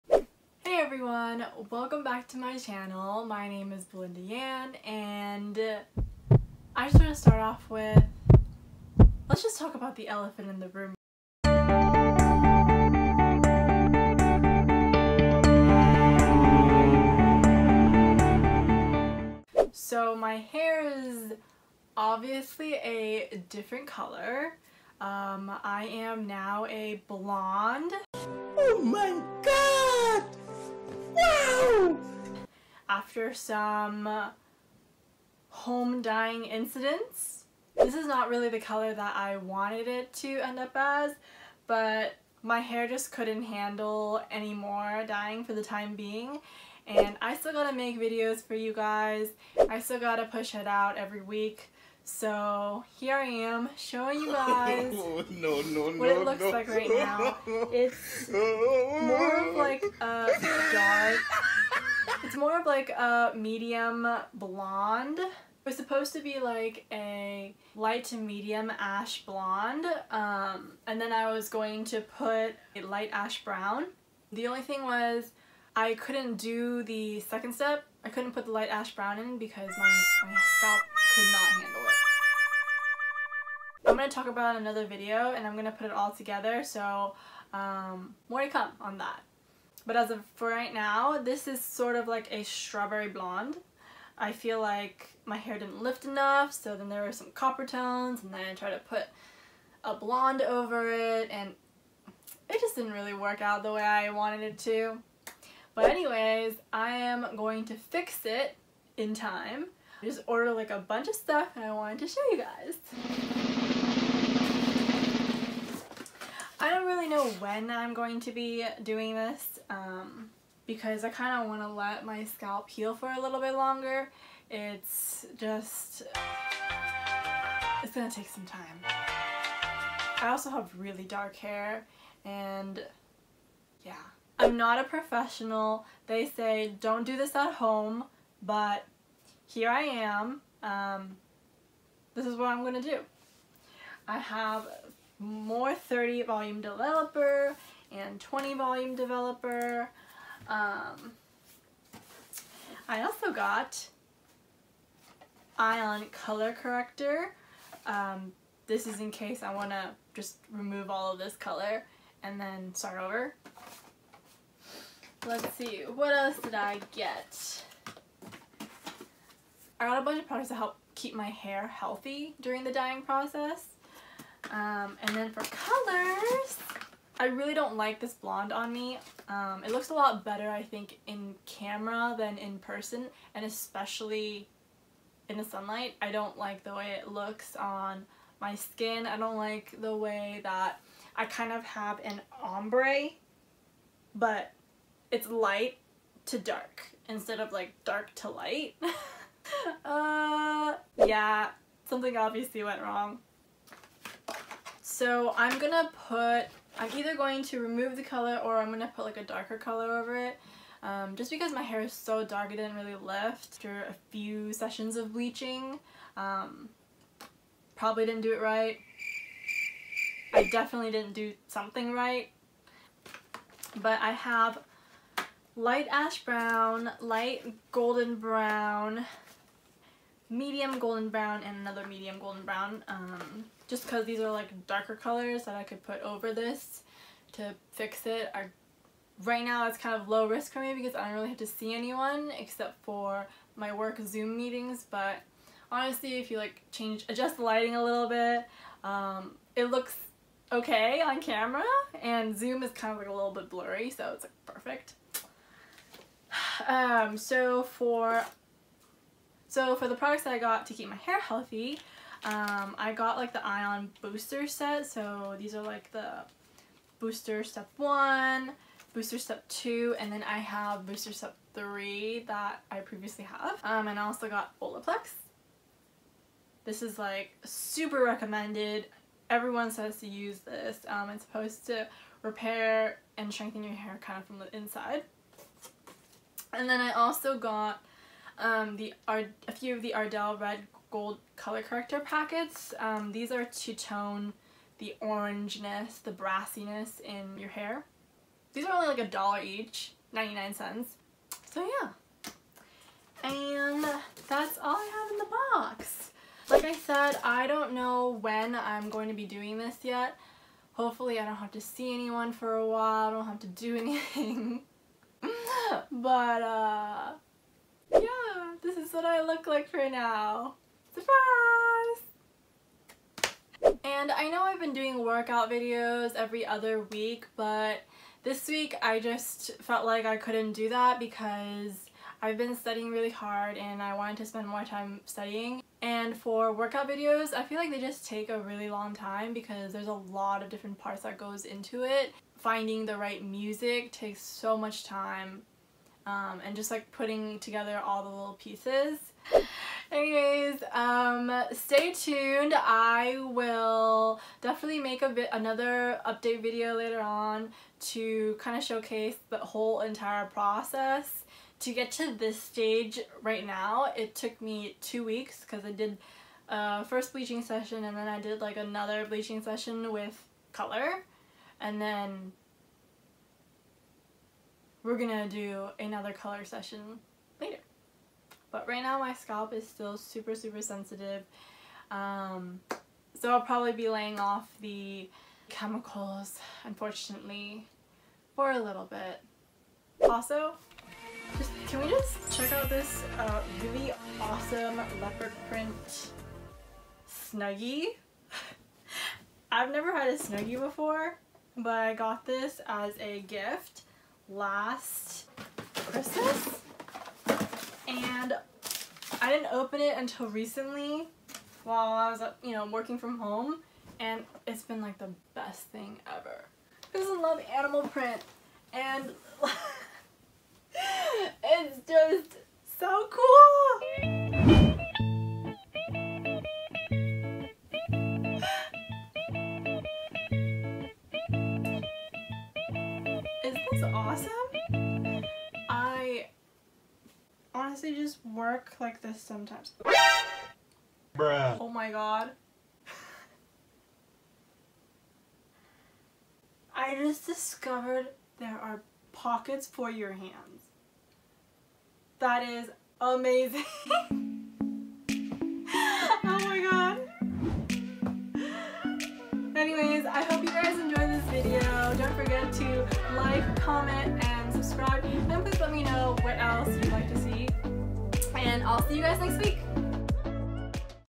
WHAT HAPPENED TO MY HAIR? (FAIL)
I tried dyeing my hair during this quarantine and this is what happened. The current color in my hair is mixture of Ion Permanent Cremes 7A/8A over a natural level 8/9 on the roots and natural level 6/7 on the ends of my hair. I’m not a fan of what it looks like right now so I bought a bunch of new stuff that just came in. I’m excited to show you guys how I plan on fixing it!!!

❤️ Belinda

☆ PRODUCTS ☆
1. 30 Volume Developer
2. 20 Volume Developer
3. Ion Color Corrector Kit
4. Ion Permanent Cremes - 4G, 5A, 5G
5. Ardell Red Gold Corrector Plus
7. Ion Step 2 Absolute Perfect Color Sealer
8. Olaplex No. 3